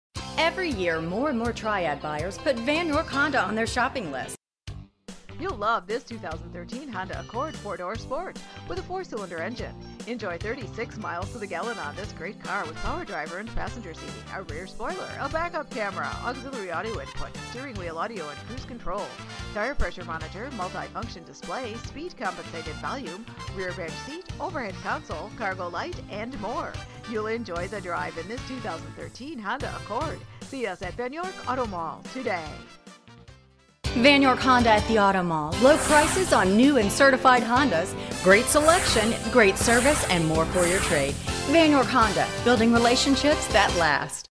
Used 2013 Honda Accord ACCORD 4DR SPORT Greensboro, Winston
New 2013 Honda Accord ACCORD 4DR SPORT Greensboro, Winston Salem High Point, Kernersville NC Vann, you will love this Basque Red Pearl Ii 2013 Honda Accord, equipped with a 4 Cyl. engine. enjoy an exceptional 36 miles to the gallon on this great car with features like power driver and passenger seating, rear spoiler, back up camera, auxiliary audio input, on steering wheel audio and cruise controls, Tire Pressure Monitoring System, multifunction display, scv - speed compensated volume, rear bench seat, and much more. Enjoy the drive and have peace of mind in this 2013 Honda Accord. see us at Vann York Auto Mall today!

vin- 1HGCR2F54DA023378

422 Eastchester Dr.